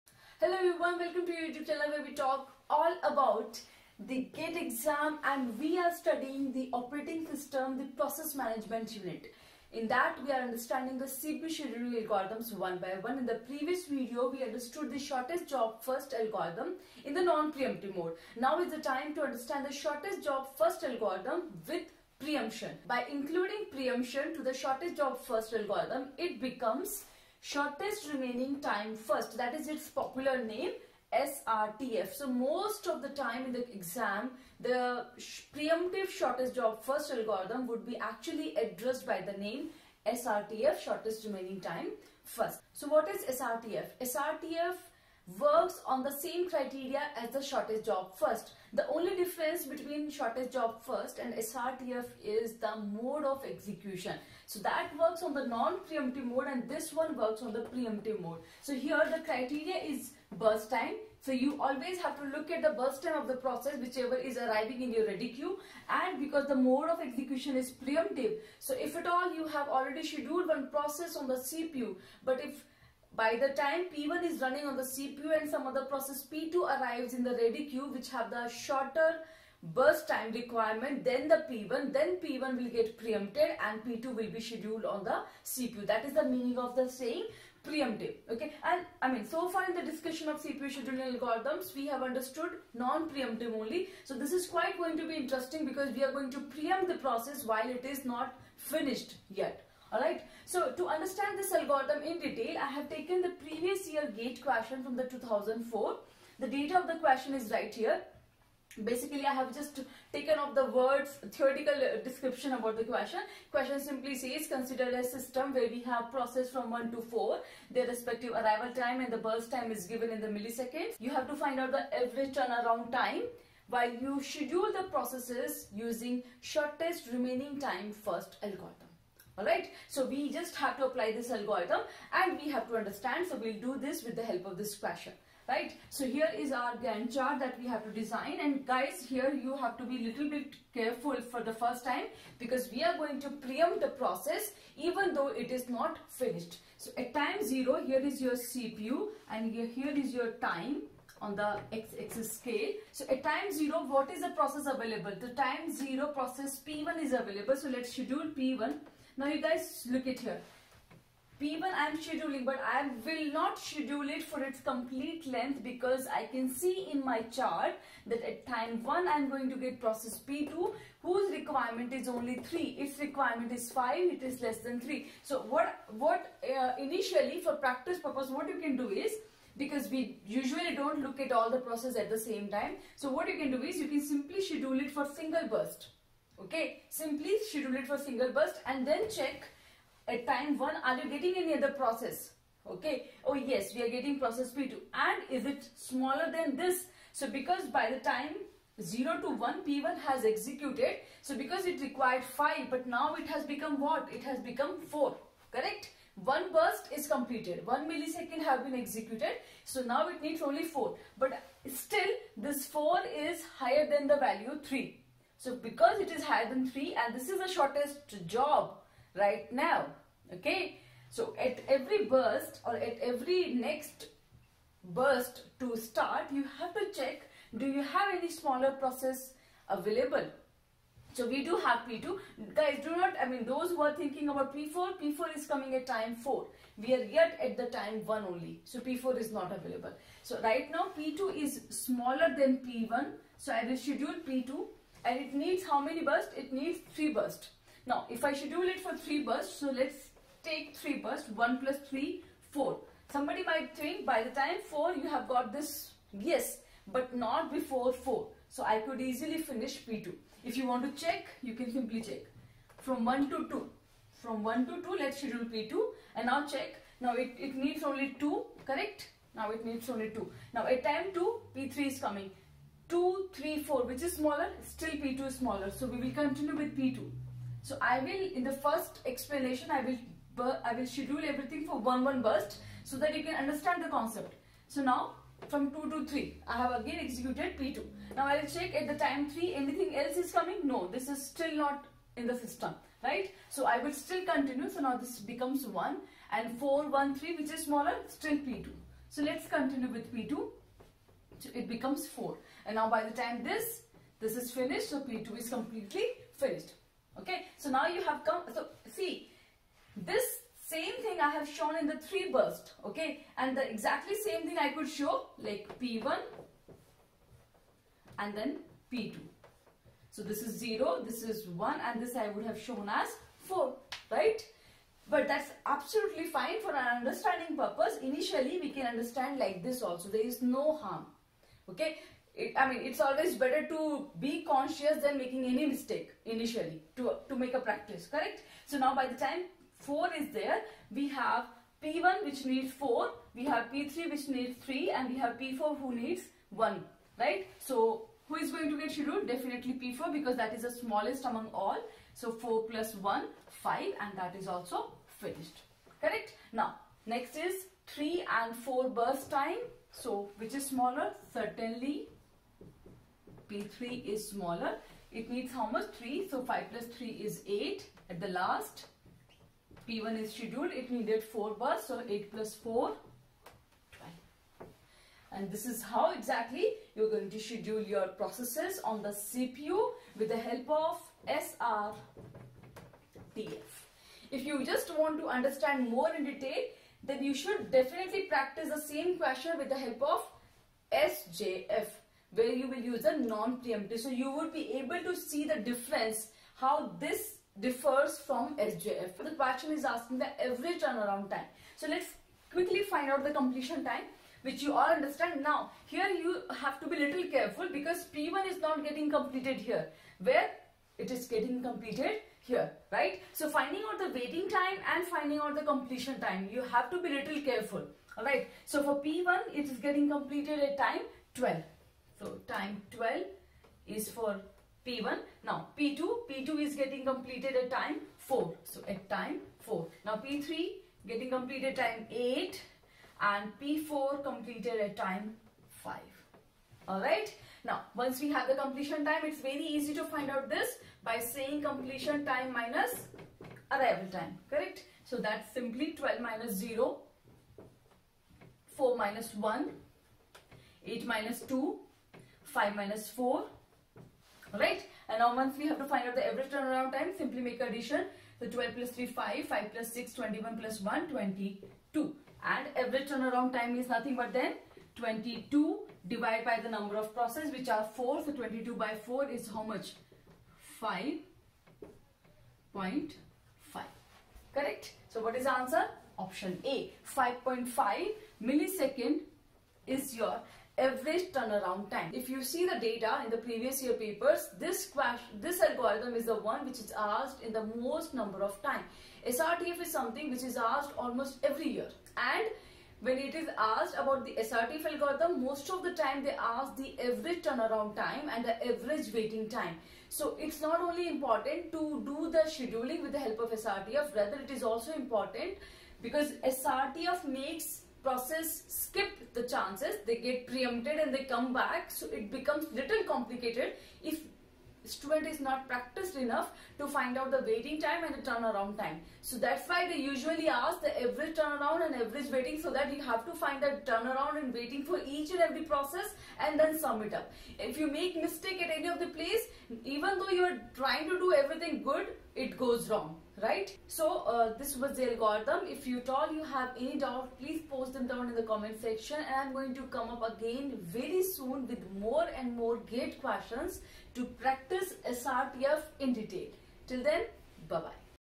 SRTF CPU Scheduling With Gate 2004 Question | Shortest Remaining Time First | OS QuickR Session 12
Time to start the Revision !! GATE Exam is right there on the corner!!
Let's start Revising each concept of Operating System with best previous year GATE questions.
It is also time to start working on QuickR (Quick Revision Notebook).
Every detail in the video.

Let's Revise every bit of it.